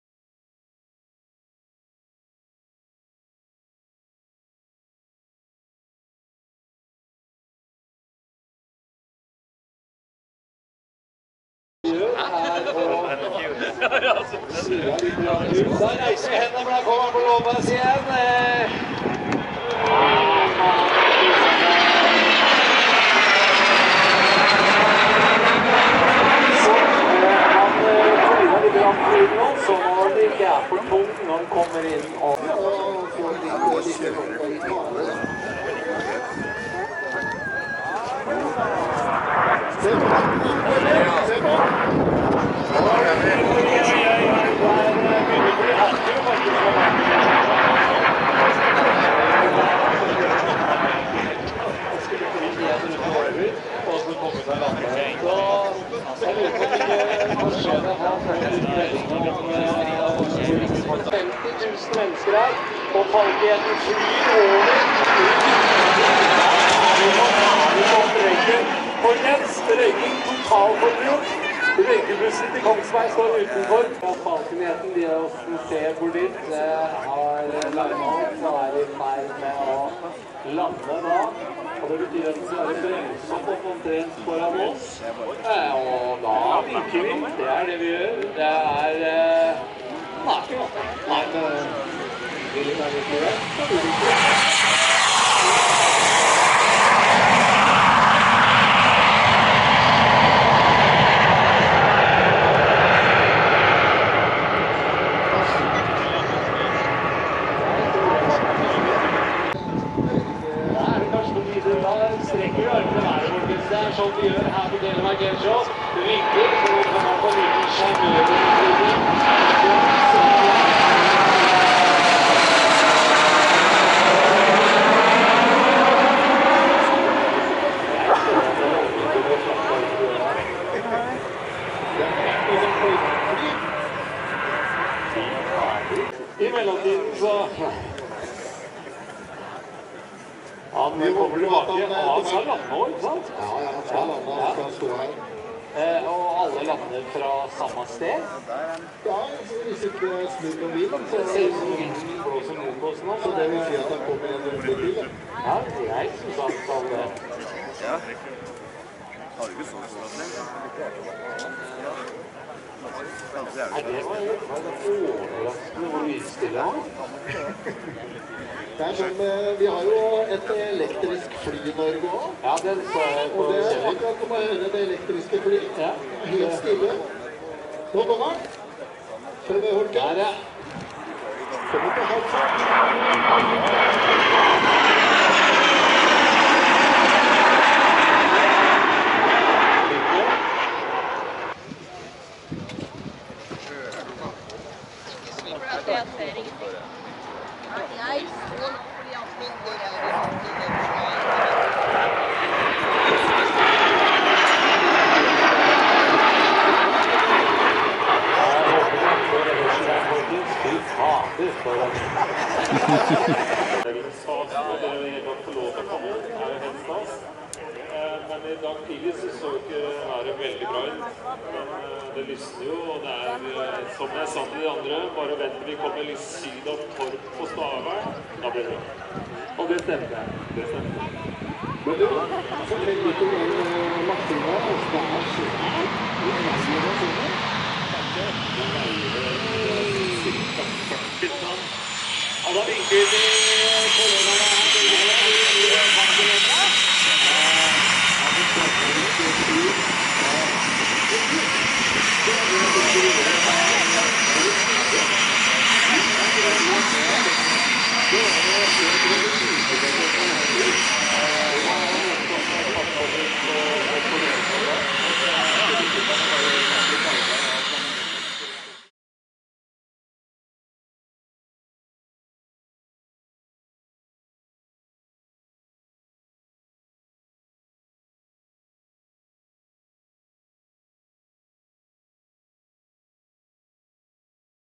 DASSAULT DA 20 FALCON JET TELEMARK AIRSHOW 2022 4K
Beutifull aircrafts last airshow before taken out off service for Norwegian airforce! Enjoy!